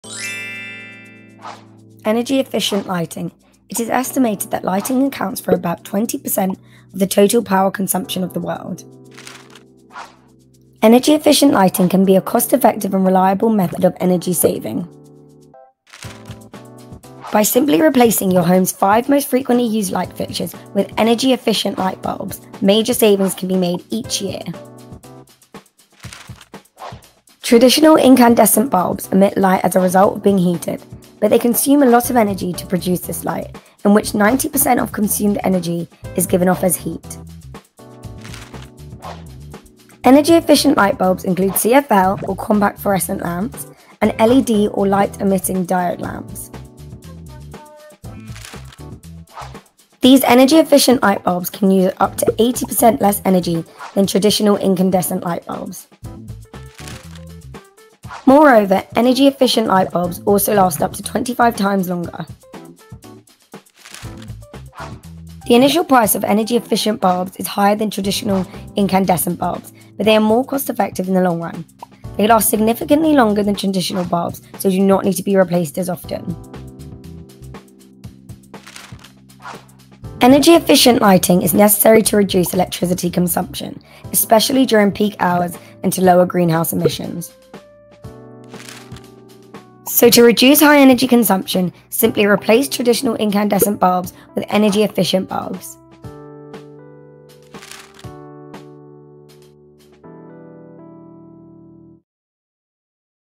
Energy-Efficient Lighting
This video highlights the importance of energy-efficient lighting. Energy efficient lighting is necessary to reduce electricity consumption and to lower greenhouse emissions. Every household, business and institution can contribute by replacing fixtures with energy efficient bulbs to save electricity.